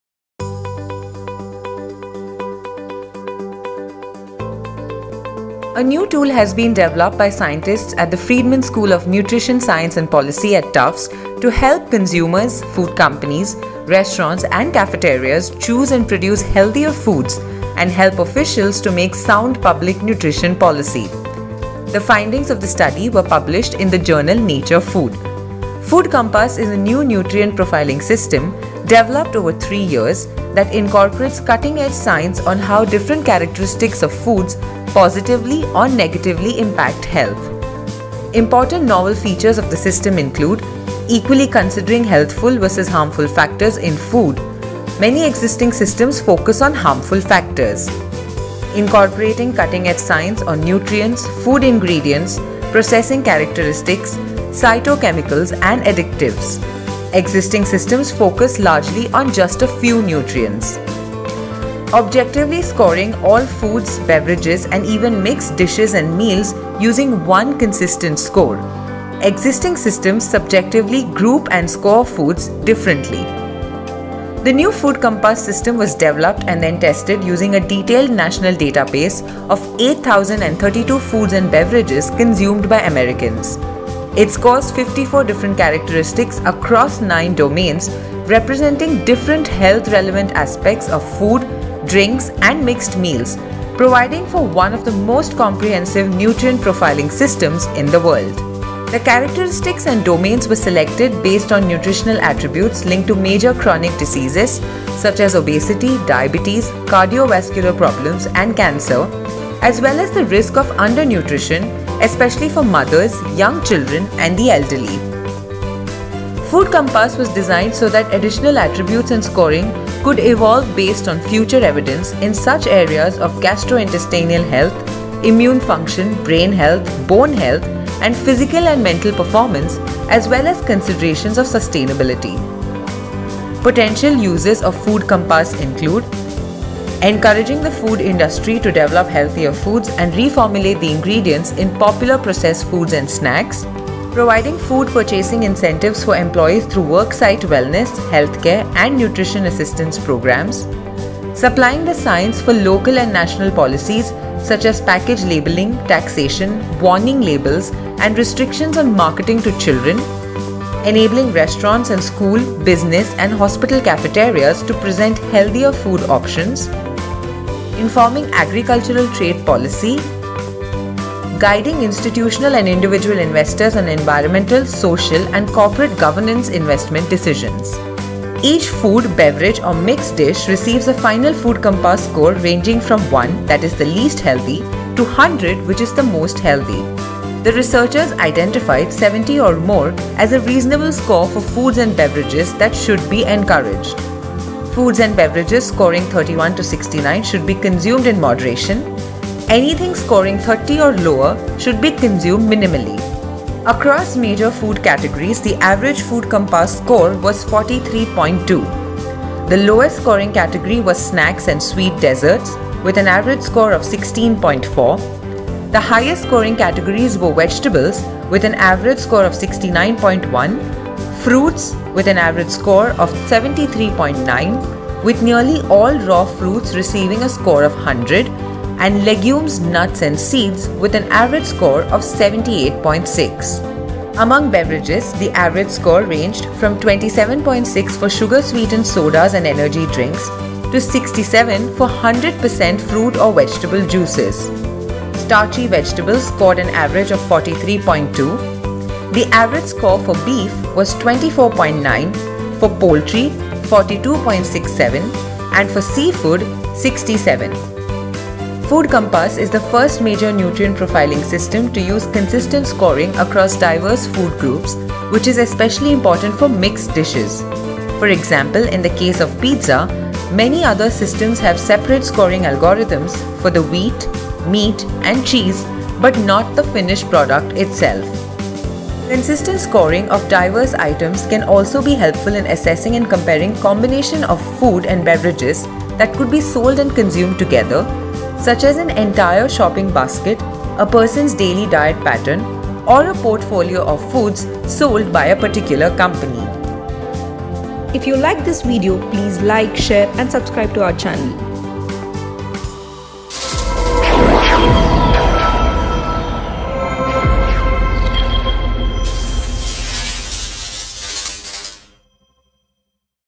Food Compass: A new tool to rate your favourite food's health quotient | Asianet Newsable
A new tool has been developed by scientists at the Friedman School of Nutrition Science and Policy at Tufts to help consumers, food companies, restaurants, and cafeterias choose and produce healthier foods and help officials to make sound public nutrition policy. The findings of the study were published in the journal 'Nature Food'.

Food Compass is a new nutrient profiling system, developed over three years, that incorporates cutting-edge science on how different characteristics of foods positively or negatively impact health.

Each food, beverage, or mixed dish receives a final Food Compass score ranging from 1 (least healthy) to 100 (most healthy).

The researchers identified 70 or more as a reasonable score for foods or beverages that should be encouraged. Foods and beverages scoring 31-69 should be consumed in moderation. Anything scoring 30 or lower should be consumed minimally.